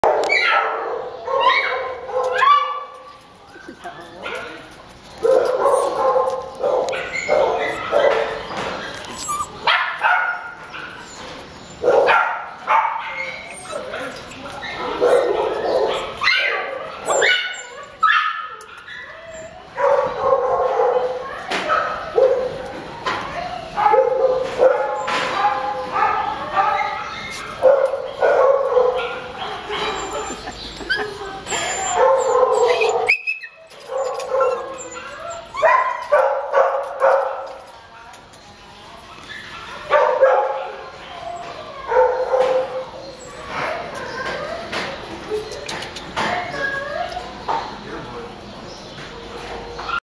Rescued!Downey A4806769 Mimsy LOVEBUG HERE!!!
A4806769
My name is Mimsy and I'm an approximately 2 year old female chihuahua sh. I am not yet spayed. I have been at the Downey Animal Care Center since March 9, 2015. I will be available on March 13, 2015. You can visit me at my temporary home at D519.